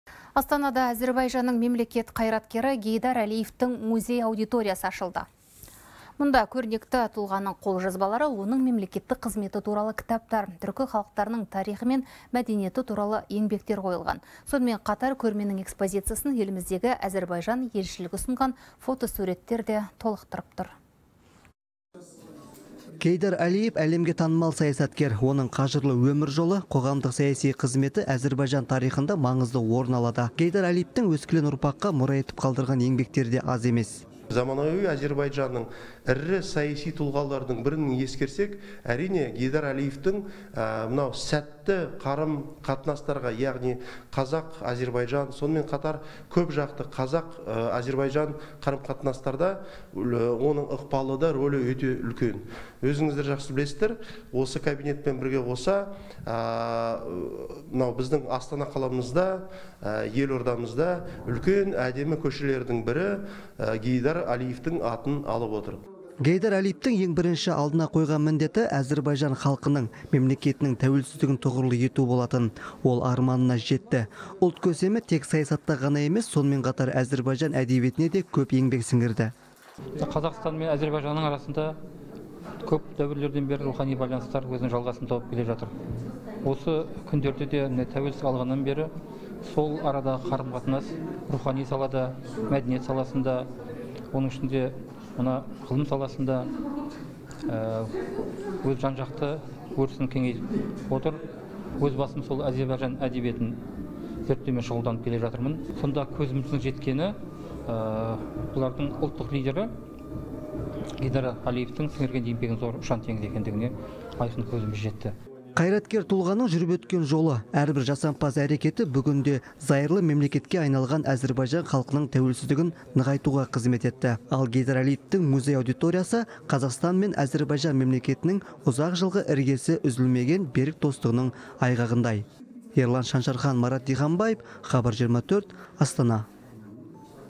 Астанада Гейдар Әлиевтің музей-аудиториясы ашылды
Астанада Әзербайжанның мемлекет қайраткері Гейдар Әлиевтің музей-аудиториясы ашылды, деп хабарлайды «Хабар 24».